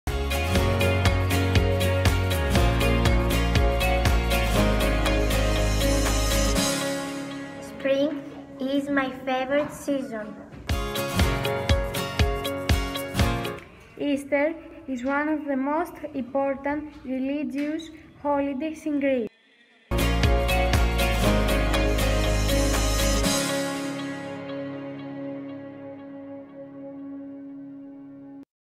ClassyVideo at school
Using the greenscreen effect with year 6 EFL learners.